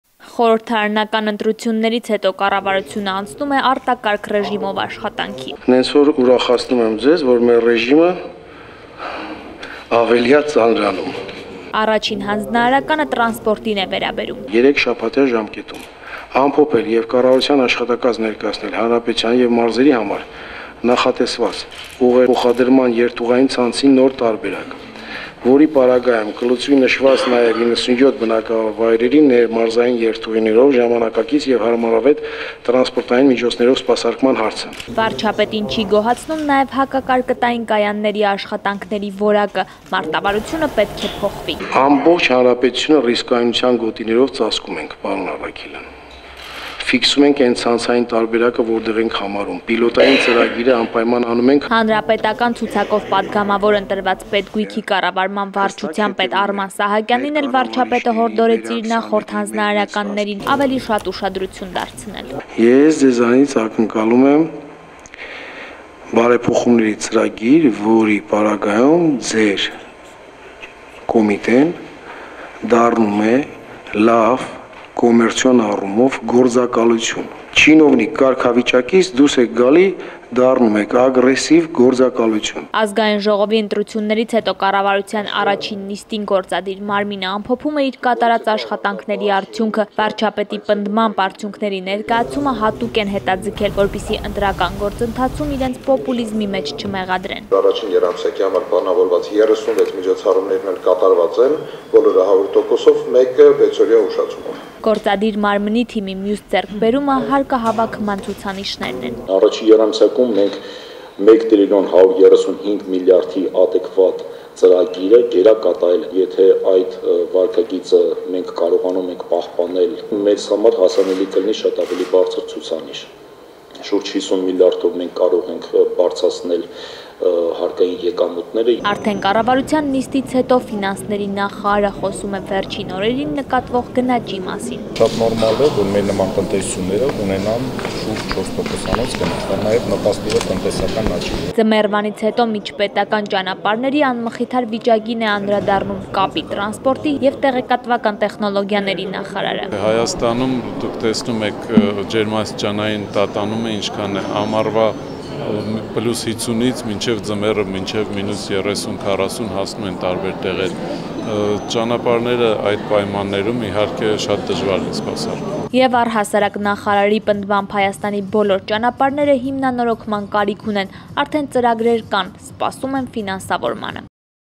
Ընտրություններից հետո նախարարը գնաճը նորմալ համարեց
Մեր թիմը ստացել է երկրում փոփոխություններ կատարելու քվեն, այսօր կառավարության նիստից առաջ հայտարարեց վարչապետ Կարեն Կարապետյանը ու հավելեց, որ գործադիր մարմնի աշխատանքային ռեժիմն այդ իսկ պատճառով դառնում է  արտակարգ:

«Աշխատանքները շարունակելու ենք նոր թափով: Ուզում եմ ուրախացնել ձեզ, մեր աշխատանքը ավելի է ծանրանալու»,- ասաց վարչապետը եւ հավելեց, որ  ապրիլի 2-ին տեղի ունեցած խորհրդարանական ընտրություններն անցել են հանգիստ եւ  հանդուրժողականության մթնոլորտում: